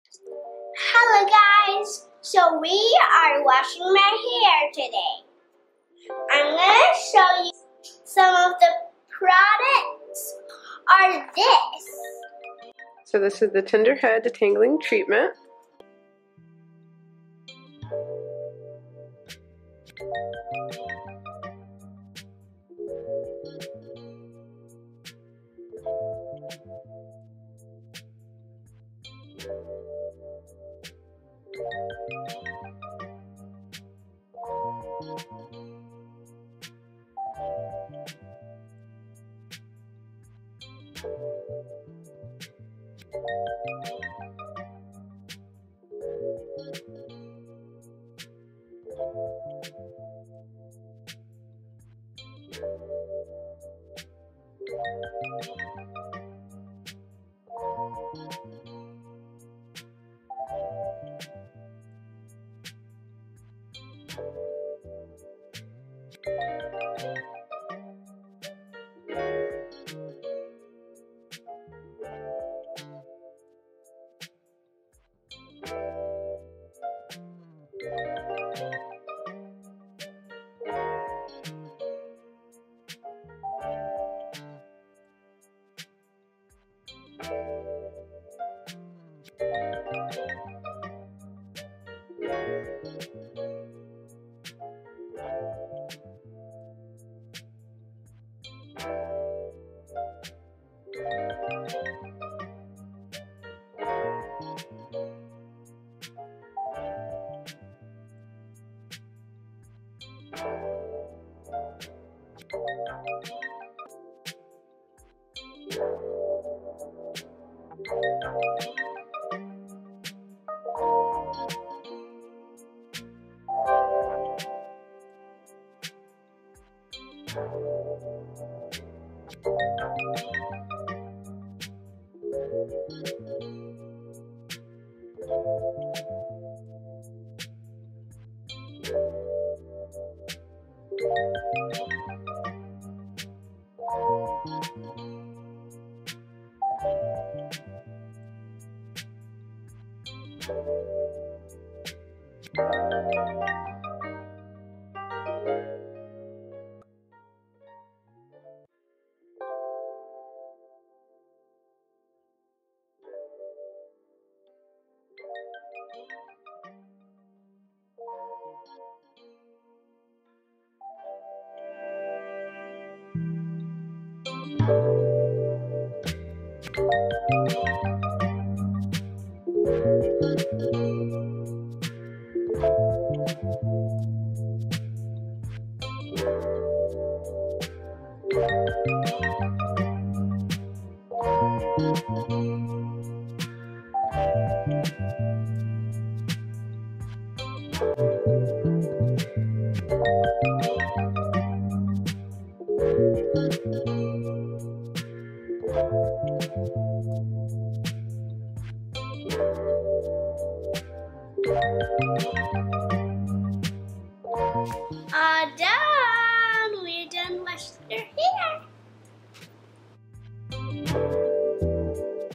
Wash Day ft. Just For Me Curl Peace
Here I am using ALL Just for Me CurlPeace products to detangle, wash, and condition her hair. The detangling treatment really smoothes the tangles away and makes the washday go by more quickly.

I use my hands to finger detangle her hair in sections with a generous amount of the detangling treatment, rinse it all out then use the Curl Peace detangling shampoo and detangling conditioner.

Detangling Treatment: